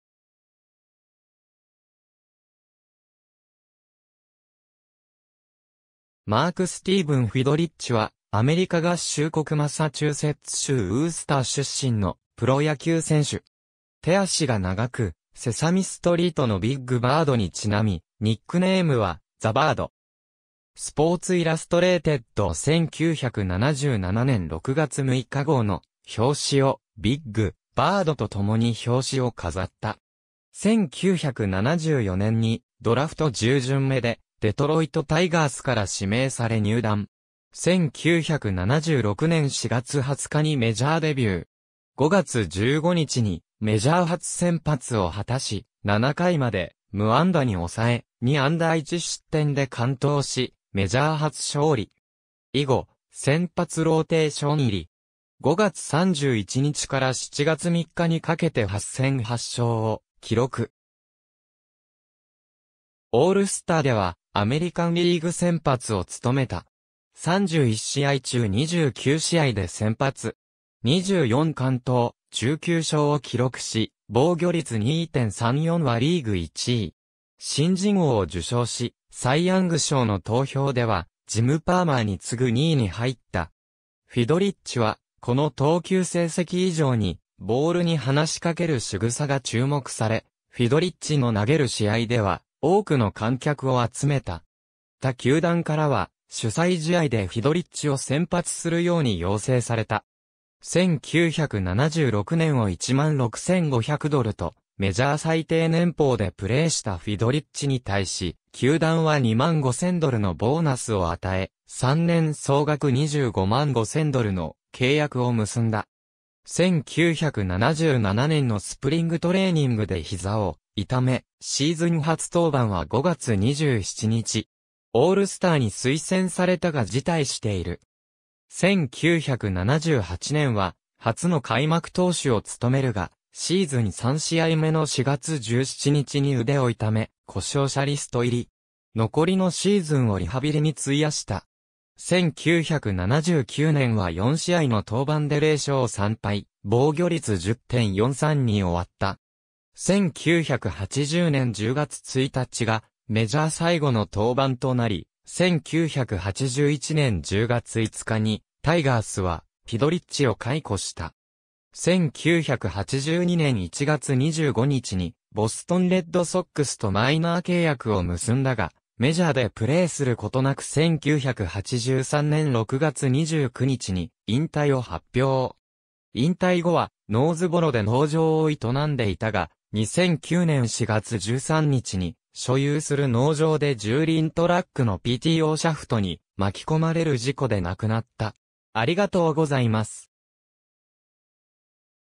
マーク・フィドリッチ
ルーキー・オブ・ザ・イヤー_(MLB)
マーク・スティーブン・フィドリッチ（Mark Steven Fidrych、1954年8月14日 - 2009年4月13日）は、アメリカ合衆国マサチューセッツ州ウースター出身のプロ野球選手（投手）。
手足が長く、セサミストリートのビッグ・バードにちなみ、ニックネームは「ザ・バード」。
スポーツ・イラストレイテッド1977年6月6日号の表紙をビッグ・バードとともに表紙を飾った。
1974年にドラフト10巡目でデトロイト・タイガースから指名され入団。
1976年4月20日にメジャーデビュー。
5月15日にメジャー初先発を果たし、7回まで無安打に抑え、2安打1失点で完投し、メジャー初勝利。
以後、先発ローテーション入り。
5月31日から7月3日にかけて8戦8勝を記録。
オールスターではアメリカンリーグ先発を務めた。
31試合中29試合で先発。
24完投、19勝を記録し、防御率2.34はリーグ1位。
新人王を受賞し、サイ・ヤング賞の投票ではジム・パーマーに次ぐ2位に入った。
フィドリッチはこの投球成績以上にボールに話しかける仕草が注目され、フィドリッチの投げる試合では多くの観客を集めた。
他球団からは主催試合でフィドリッチを先発するように要請された。
1976年を1万6,500ドルとメジャー最低年俸でプレイしたフィドリッチに対し、球団は2万5,000ドルのボーナスを与え、3年総額25万5,000ドルの契約を結んだ。
1977年のスプリング・トレーニングで膝を痛め、シーズン初登板は5月27日。
オールスターに推薦されたが辞退している。
1978年は初の開幕投手を務めるが、シーズン3試合目の4月17日に腕を痛め、故障者リスト入り。
残りのシーズンをリハビリに費やした。
1979年は4試合の登板で0勝3敗・防御率10.43に終わった。
1980年10月1日がメジャー最後の登板となり、1981年10月5日にタイガースはフィドリッチを解雇した。
1982年1月25日にボストン・レッドソックスとマイナー契約を結んだが、メジャーでプレイすることなく1983年6月29日に引退を発表。
引退後はノーズボロで農場を営んでいたが、2009年4月13日に、所有する農場で10輪トラックのPTOシャフトに巻き込まれる事故で亡くなった。